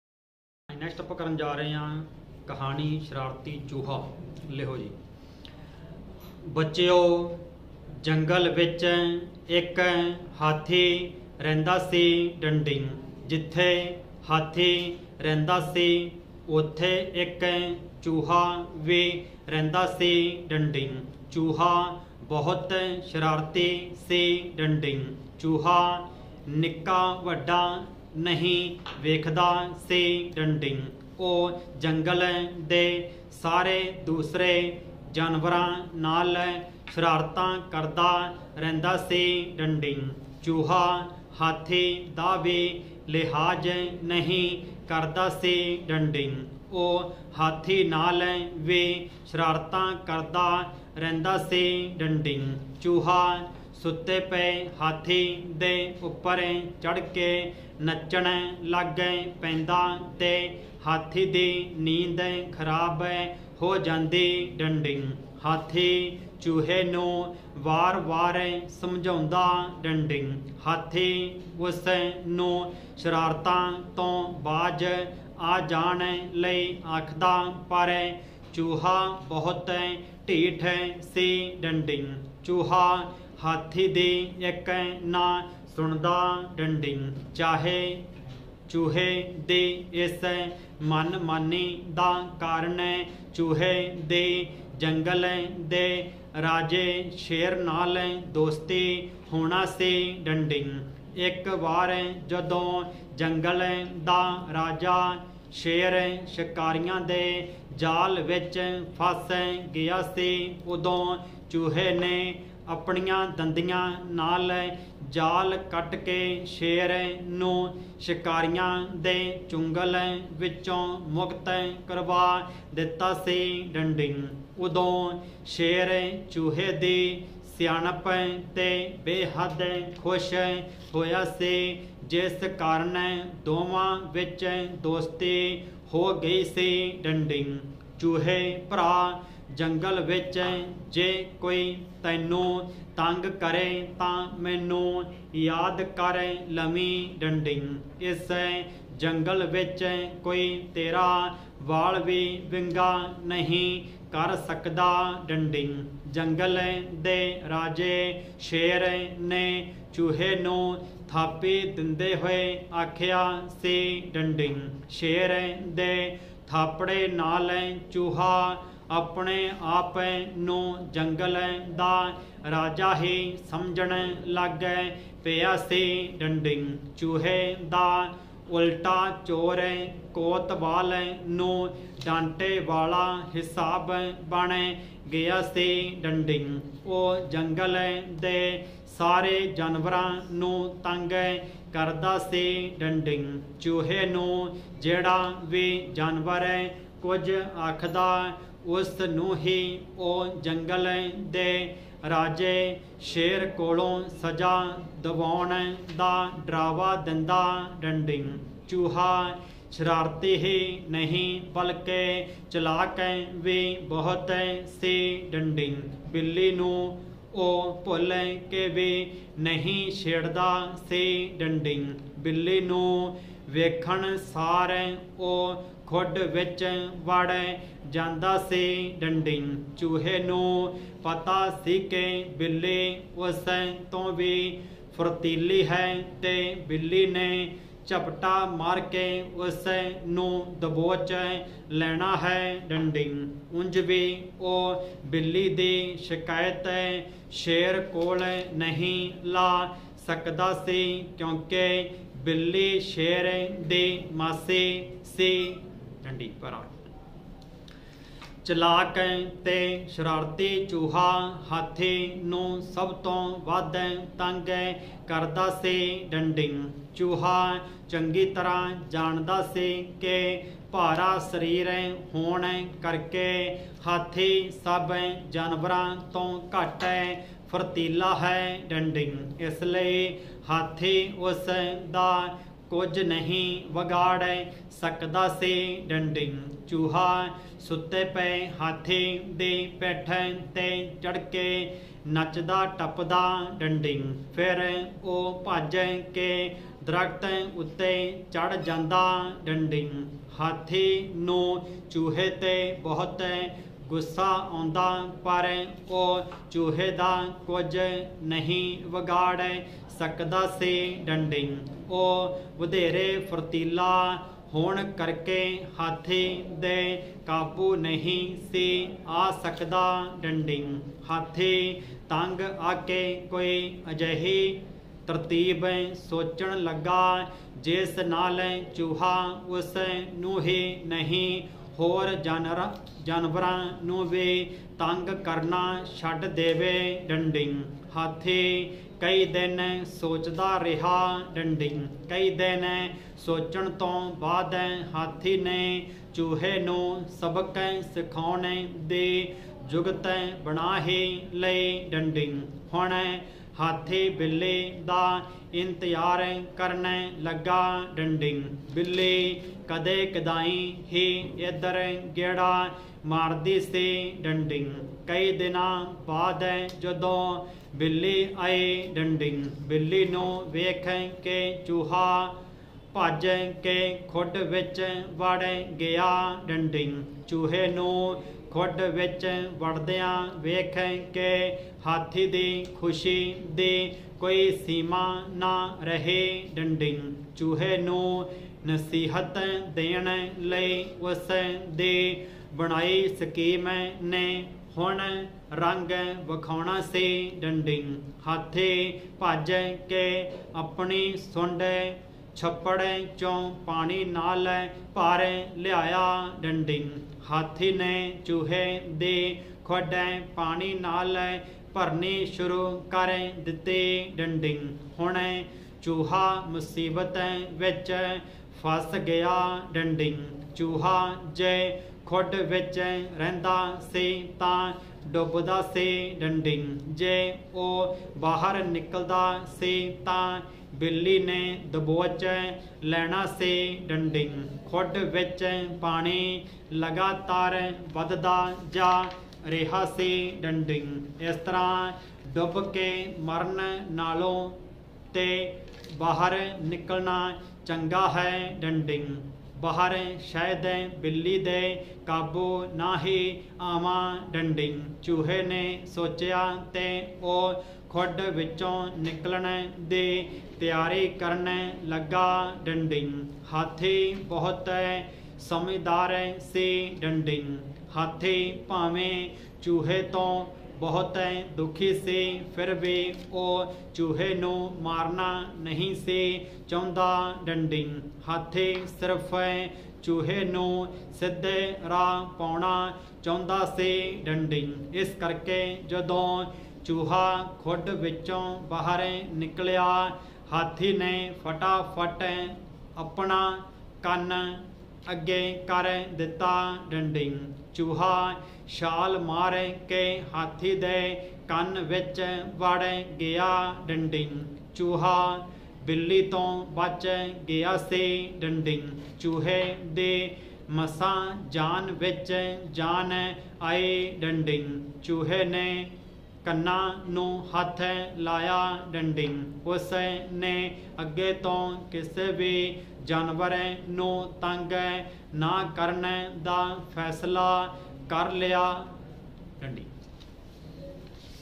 ਸ਼ਰਾਰਤੀ ਚੂਹਾ Story Skill Test PSSSB May 2023 Shorthand Punjabi Dictation By Sukhwinder Singh Sherpuri
ਸ਼ਰਾਰਤੀ ਚੂਹਾ (ਕਹਾਣੀ) : ਇਕਬਾਲ ਸਿੰਘ ਹਮਜਾਪੁਰ
ਬੱਚਿਓ! ਜੰਗਲ ਵਿਚ ਇਕ ਹਾਥੀ ਰਹਿੰਦਾ ਸੀ। ਜਿੱਥੇ ਹਾਥੀ ਰਹਿੰਦਾ ਸੀ, ਉੱਥੇ ਇਕ ਚੂਹਾ ਵੀ ਰਹਿੰਦਾ ਸੀ। ਚੂਹਾ ਬਹੁਤ ਸ਼ਰਾਰਤੀ ਸੀ। ਚੂਹਾ ਨਿੱਕਾ-ਵੱਡਾ ਨਹੀਂ ਵੇਖਦਾ ਸੀ। ਉਹ ਜੰਗਲ ਦੇ ਸਾਰੇ ਦੂਸਰੇ ਜਾਨਵਰਾਂ ਨਾਲ ਸ਼ਰਾਰਤਾਂ ਕਰਦਾ ਰਹਿੰਦਾ ਸੀ। ਚੂਹਾ ਹਾਥੀ ਦਾ ਵੀ ਲਿਹਾਜ ਨਹੀਂ ਕਰਦਾ ਸੀ। ਉਹ ਹਾਥੀ ਨਾਲ ਵੀ ਸ਼ਰਾਰਤਾਂ ਕਰਦਾ ਰਹਿੰਦਾ ਸੀ। ਚੂਹਾ ਸੁੱਤੇ ਪਏ ਹਾਥੀ ਦੇ ਉੱਪਰ ਚੜ੍ਹ ਕੇ ਨੱਚਣ ਲੱਗ ਪੈਂਦਾ ਤੇ ਹਾਥੀ ਦੀ ਨੀਂਦ ਖਰਾਬ ਹੋ ਜਾਂਦੀ। ਹਾਥੀ, ਚੂਹੇ ਨੂੰ ਵਾਰ ਵਾਰ ਸਮਝਾਉਂਦਾ। ਹਾਥੀ ਉਸ ਨੂੰ ਸ਼ਰਾਰਤਾਂ ਤੋਂ ਬਾਜ਼ ਆ ਜਾਣ ਲਈ ਆਖਦਾ, ਪਰ ਚੂਹਾ ਬਹੁਤ ਢੀਠ ਸੀ। ਚੂਹਾ, ਹਾਥੀ ਦੀ ਇਕ ਨਾ ਸੁਣਦਾ। ਚੂਹੇ ਦੀ ਇਸ ਮਨਮਾਨੀ ਦਾ ਕਾਰਨ, ਚੂਹੇ ਦੀ ਜੰਗਲ ਦੇ ਰਾਜੇ ਸ਼ੇਰ ਨਾਲ ਦੋਸਤੀ ਹੋਣਾ ਸੀ।
ਇਕ ਵਾਰ ਜਦੋਂ ਜੰਗਲ ਦਾ ਰਾਜਾ ਸ਼ੇਰ ਸ਼ਿਕਾਰੀਆਂ ਦੇ ਜਾਲ ਵਿਚ ਫਸ ਗਿਆ ਸੀ, ਉਦੋਂ ਚੂਹੇ ਨੇ ਆਪਣੀਆਂ ਦੰਦੀਆਂ ਨਾਲ ਜਾਲ ਕੱਟ ਕੇ ਸ਼ੇਰ ਨੂੰ ਸ਼ਿਕਾਰੀਆਂ ਦੇ ਚੁੰਗਲ ਵਿਚੋਂ ਮੁਕਤ ਕਰਵਾ ਦਿੱਤਾ ਸੀ। ਉਦੋਂ ਸ਼ੇਰ, ਚੂਹੇ ਦੀ ਸਿਆਣਪ ’ਤੇ ਬੇਹੱਦ ਖੁਸ਼ ਹੋਇਆ ਸੀ ਜਿਸ ਕਾਰਨ ਦੋਵਾਂ ਵਿਚ ਦੋਸਤੀ ਹੋ ਗਈ ਸੀ।
‘ਚੂਹੇ ਭਰਾ! ਜੰਗਲ ਵਿਚ ਜੇ ਕੋਈ ਤੈਨੂੰ ਤੰਗ ਕਰੇ ਤਾਂ ਮੈਨੂੰ ਯਾਦ ਕਰ ਲਵੀਂ। ਇਸ ਜੰਗਲ ਵਿਚ ਕੋਈ ਤੇਰਾ ਵਾਲ ਵੀ ਵਿੰਗਾ ਨਹੀਂ ਕਰ ਸਕਦਾ।’ ਜੰਗਲ ਦੇ ਰਾਜੇ ਸ਼ੇਰ ਨੇ ਚੂਹੇ ਨੂੰ ਥਾਪੀ ਦਿੰਦੇ ਹੋਏ ਆਖਿਆ ਸੀ।
ਸ਼ੇਰ ਦੇ ਥਾਪੜੇ ਨਾਲ ਚੂਹਾ ਆਪਣੇ-ਆਪ ਨੂੰ ਜੰਗਲ ਦਾ ਰਾਜਾ ਹੀ ਸਮਝਣ ਲੱਗ ਪਿਆ ਸੀ। ਚੂਹੇ ਦਾ ‘ਉਲਟਾ ਚੋਰ ਕੋਤਵਾਲ ਨੂੰ ਡਾਂਟੇ’ ਵਾਲਾ ਹਿਸਾਬ ਬਣ ਗਿਆ ਸੀ। ਉਹ ਜੰਗਲ ਦੇ ਸਾਰੇ ਜਾਨਵਰਾਂ ਨੂੰ ਤੰਗ ਕਰਦਾ ਸੀ। ਚੂਹੇ ਨੂੰ ਜਿਹੜਾ ਵੀ ਜਾਨਵਰ ਕੁਝ ਆਖਦਾ, ਉਸ ਨੂੰ ਹੀ ਉਹ ਜੰਗਲ ਦੇ ਰਾਜੇ ਸ਼ੇਰ ਕੋਲੋਂ ਸਜ਼ਾ ਦਿਵਾਉਣ ਦਾ ਡਰਾਵਾ ਦਿੰਦਾ।
ਚੂਹਾ ਸ਼ਰਾਰਤੀ ਹੀ ਨਹੀਂ ਬਲਕਿ ਚਲਾਕ ਵੀ ਬਹੁਤ ਸੀ। ਬਿੱਲੀ ਨੂੰ ਉਹ ਭੁੱਲ ਕੇ ਵੀ ਨਹੀਂ ਛੇੜਦਾ ਸੀ। ਬਿੱਲੀ ਨੂੰ ਵੇਖਣ ਸਾਰ ਉਹ ਖੁੱਡ ਵਿਚ ਵੜ ਜਾਂਦਾ ਸੀ। ਚੂਹੇ ਨੂੰ ਪਤਾ ਸੀ ਕਿ ਬਿੱਲੀ ਉਸ ਤੋਂ ਵੀ ਫੁਰਤੀਲੀ ਹੈ ਤੇ ਬਿੱਲੀ ਨੇ ਝਪਟਾ ਮਾਰ ਕੇ ਉਸ ਨੂੰ ਦਬੋਚ ਲੈਣਾ ਹੈ। ਉਂਜ ਵੀ ਉਹ ਬਿੱਲੀ ਦੀ ਸ਼ਿਕਾਇਤ ਸ਼ੇਰ ਕੋਲ ਨਹੀਂ ਲਾ ਸਕਦਾ ਸੀ ਕਿਉਂਕਿ ਬਿੱਲੀ ਸ਼ੇਰ ਦੀ ਮਾਸੀ ਸੀ।
ਚਲਾਕ ਤੇ ਸ਼ਰਾਰਤੀ ਚੂਹਾ, ਹਾਥੀ ਨੂੰ ਸਭ ਤੋਂ ਵੱਧ ਤੰਗ ਕਰਦਾ ਸੀ। ਚੂਹਾ ਚੰਗੀ ਤਰ੍ਹਾਂ ਜਾਣਦਾ ਸੀ ਕਿ ਭਾਰਾ ਸਰੀਰ ਹੋਣ ਕਰਕੇ ਹਾਥੀ ਸਭ ਜਾਨਵਰਾਂ ਤੋਂ ਘੱਟ ਫੁਰਤੀਲਾ ਹੈ। ਇਸ ਲਈ ਹਾਥੀ ਉਸ ਦਾ ਕੁਝ ਨਹੀਂ ਵਿਗਾੜ ਸਕਦਾ ਸੀ। ਚੂਹਾ ਸੁੱਤੇ ਪਏ ਹਾਥੀ ਦੀ ਪਿੱਠ ’ਤੇ ਚੜ੍ਹਕੇ ਨੱਚਦਾ-ਟੱਪਦਾ। ਫਿਰ ਉਹ ਭੱਜ ਕੇ ਦਰੱਖਤ ਉੱਤੇ ਚੜ੍ਹ ਜਾਂਦਾ। ਹਾਥੀ ਨੂੰ ਚੂਹੇ ’ਤੇ ਬਹੁਤ ਗੁੱਸਾ ਆਉਂਦਾ, ਪਰ ਉਹ ਚੂਹੇ ਦਾ ਕੁਝ ਨਹੀਂ ਵਿਗਾੜ ਸਕਦਾ ਸੀ। ਉਹ ਵਧੇਰੇ ਫੁਰਤੀਲਾ ਹੋਣ ਕਰਕੇ ਹਾਥੀ ਦੇ ਕਾਬੂ ਨਹੀਂ ਸੀ ਆ ਸਕਦਾ। ਹਾਥੀ ਤੰਗ ਆ ਕੇ ਕੋਈ ਅਜਿਹੀ ਤਰਤੀਬ ਸੋਚਣ ਲੱਗਾ ਜਿਸ ਨਾਲ ਚੂਹਾ ਉਸ ਨੂੰ ਹੀ ਨਹੀਂ, ਹੋਰ ਜਾਨਵਰਾਂ ਨੂੰ ਵੀ ਤੰਗ ਕਰਨਾ ਛੱਡ ਦੇਵੇ। ਹਾਥੀ ਕਈ ਦਿਨ ਸੋਚਦਾ ਰਿਹਾ। ਕਈ ਦਿਨ ਸੋਚਣ ਤੋਂ ਬਾਅਦ ਹਾਥੀ ਨੇ ਚੂਹੇ ਨੂੰ ਸਬਕ ਸਿਖਾਉਣ ਦੀ ਜੁਗਤ ਬਣਾ ਹੀ ਲਈ।
ਹੁਣ ਹਾਥੀ ਬਿੱਲੀ ਦਾ ਇੰਤਜ਼ਾਰ ਕਰਨ ਲੱਗਾ। ਬਿੱਲੀ ਕਦੇ-ਕਦਾਈਂ ਹੀ ਇਧਰ ਗੇੜਾ ਮਾਰਦੀ ਸੀ। ਕਈ ਦਿਨਾਂ ਬਾਅਦ ਜਦੋਂ ਬਿੱਲੀ ਆਈ। ਬਿੱਲੀ ਨੂੰ ਵੇਖ ਕੇ ਚੂਹਾ ਭੱਜ ਕੇ ਖੁੱਡ ਵਿਚ ਵੜ ਗਿਆ। ਚੂਹੇ ਨੂੰ ਖੁੱਡ ਵਿਚ ਵੜਦਿਆਂ ਵੇਖ ਕੇ ਹਾਥੀ ਦੀ ਖੁਸ਼ੀ ਦੀ ਕੋਈ ਸੀਮਾ ਨਾ ਰਹੀ। ਚੂਹੇ ਨੂੰ ਨਸੀਹਤ ਦੇਣ ਲਈ ਉਸ ਦੀ ਬਣਾਈ ਸਕੀਮ ਨੇ ਹੁਣ ਰੰਗ ਵਿਖਾਉਣਾ ਸੀ।
ਹਾਥੀ ਭੱਜ ਕੇ ਆਪਣੀ ਸੁੰਡ ਛੱਪੜ ’ਚੋਂ ਪਾਣੀ ਨਾਲ ਭਰ ਲਿਆਇਆ। ਹਾਥੀ ਨੇ ਚੂਹੇ ਦੀ ਖੁੱਡ ਪਾਣੀ ਨਾਲ ਭਰਨੀ ਸ਼ੁਰੂ ਕਰ ਦਿੱਤੀ। ਹੁਣ ਚੂਹਾ ਮੁਸੀਬਤ ਵਿਚ ਫਸ ਗਿਆ। ਚੂਹਾ ਜੇ ਖੁੱਡ ਵਿਚ ਰਹਿੰਦਾ ਸੀ ਤਾਂ ਡੁੱਬਦਾ ਸੀ। ਜੇ ਉਹ ਬਾਹਰ ਨਿਕਲਦਾ ਸੀ ਤਾਂ ਬਿੱਲੀ ਨੇ ਦਬੋਚ ਲੈਣਾ ਸੀ। ਖੁੱਡ ਵਿਚ ਪਾਣੀ ਲਗਾਤਾਰ ਵਧਦਾ ਜਾ ਰਿਹਾ ਸੀ।
‘ਇਸ ਤਰ੍ਹਾਂ ਡੁੱਬ ਕੇ ਮਰਨ ਨਾਲੋਂ ਤੇ ਬਾਹਰ ਨਿਕਲਣਾ ਚੰਗਾ ਹੈ। ਬਾਹਰ ਸ਼ਾਇਦ ਬਿੱਲੀ ਦੇ ਕਾਬੂ ਨਾ ਹੀ ਆਵਾਂ।’ ਚੂਹੇ ਨੇ ਸੋਚਿਆ ਤੇ ਉਹ ਖੁੱਡ ਵਿਚੋਂ ਨਿਕਲਣ ਦੀ ਤਿਆਰੀ ਕਰਨ ਲੱਗਾ।
ਹਾਥੀ ਬਹੁਤ ਸਮਝਦਾਰ ਸੀ। ਹਾਥੀ ਭਾਵੇਂ ਚੂਹੇ ਤੋਂ ਬਹੁਤ ਦੁਖੀ ਸੀ, ਫਿਰ ਵੀ ਉਹ ਚੂਹੇ ਨੂੰ ਮਾਰਨਾ ਨਹੀਂ ਸੀ ਚਾਹੁੰਦਾ। ਹਾਥੀ ਸਿਰਫ਼ ਚੂਹੇ ਨੂੰ ਸਿੱਧੇ ਰਾਹ ਪਾਉਣਾ ਚਾਹੁੰਦਾ ਸੀ। ਇਸ ਕਰਕੇ ਜਦੋਂ ਚੂਹਾ ਖੁੱਡ ਵਿਚੋਂ ਬਾਹਰ ਨਿਕਲਿਆ, ਹਾਥੀ ਨੇ ਫਟਾਫਟ ਆਪਣਾ ਕੰਨ ਅੱਗੇ ਕਰ ਦਿੱਤਾ। ਚੂਹਾ ਛਾਲ ਮਾਰ ਕੇ ਹਾਥੀ ਦੇ ਕੰਨ ਵਿਚ ਵੜ ਗਿਆ।
ਚੂਹਾ, ਬਿੱਲੀ ਤੋਂ ਬਚ ਗਿਆ ਸੀ। ਚੂਹੇ ਦੀ ਮਸ੍ਵਾਂ ਜਾਨ ਵਿਚ ਜਾਨ ਆਈ। ਚੂਹੇ ਨੇ ਕੰਨਾਂ ਨੂੰ ਹੱਥ ਲਾਇਆ। ਉਸ ਨੇ ਅੱਗੇ ਤੋਂ ਕਿਸੇ ਵੀ ਜਾਨਵਰ ਨੂੰ ਤੰਗ ਨਾ ਕਰਨ ਦਾ ਫ਼ੈਸਲਾ ਕਰ ਲਿਆ।